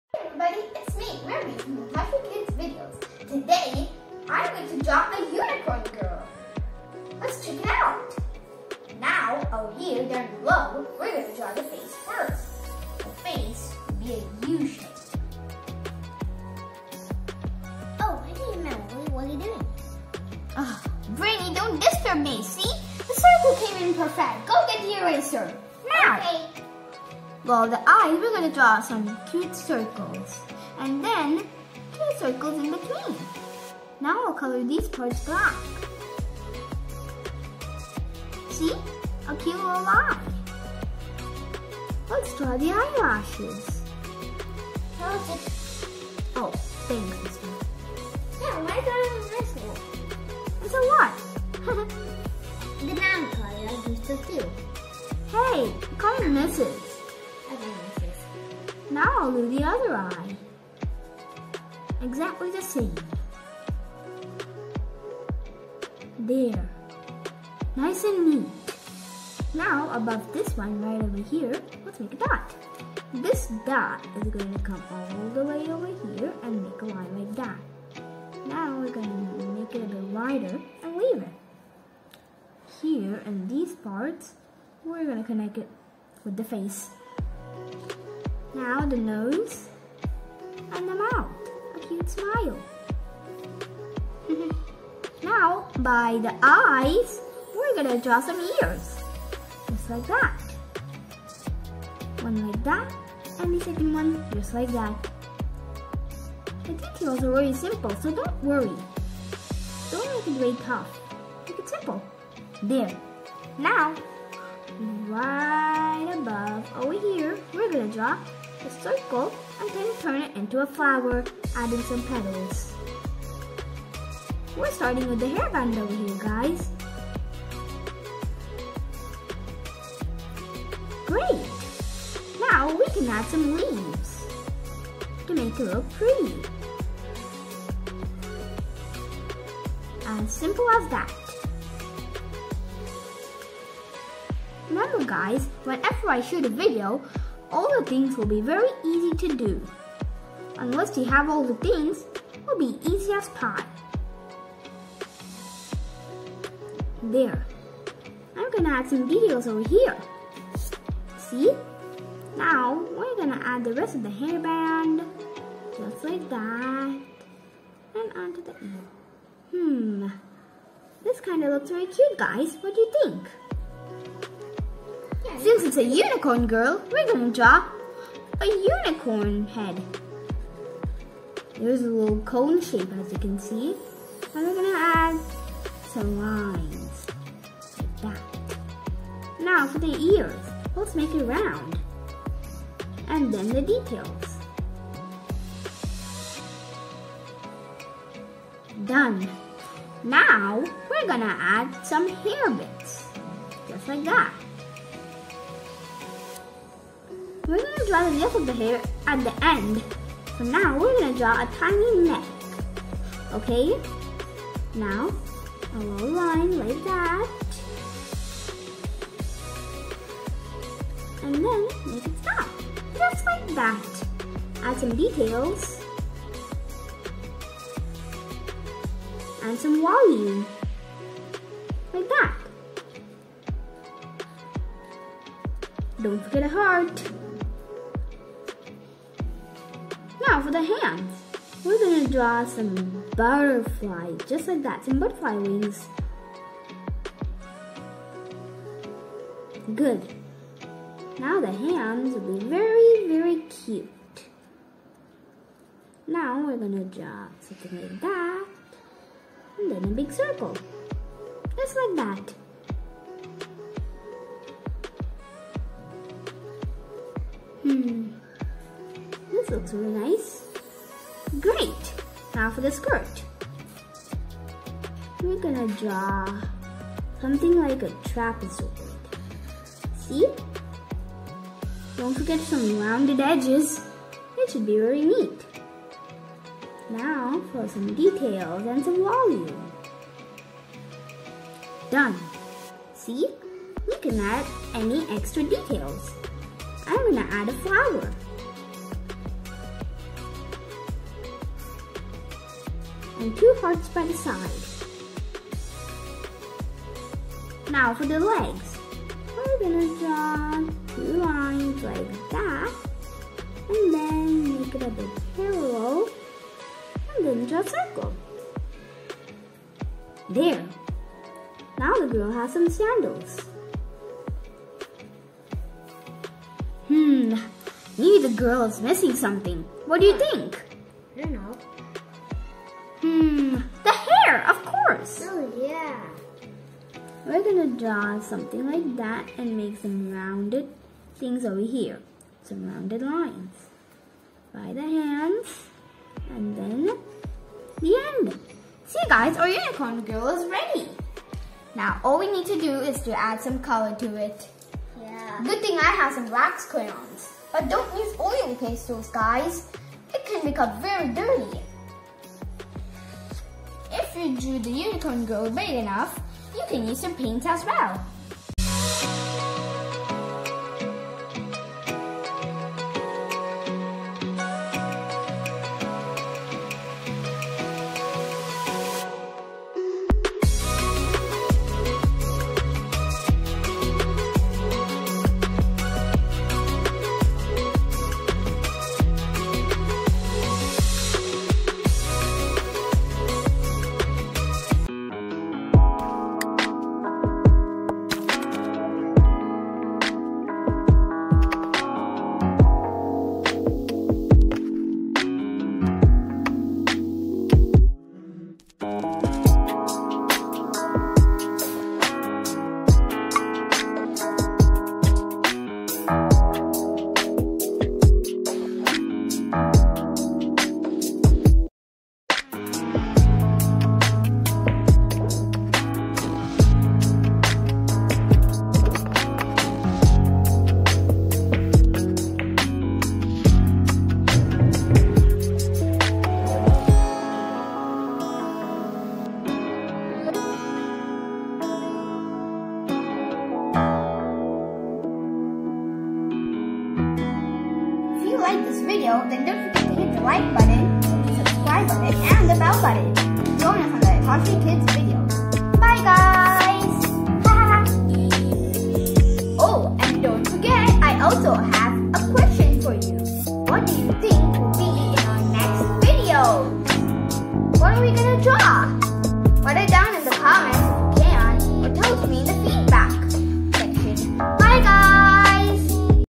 DIY ||How to draw unicorn girl|| for kids in summer time easy drawing
How to draw unicorn girl by easy steps for kids in summer time

follow me how i drew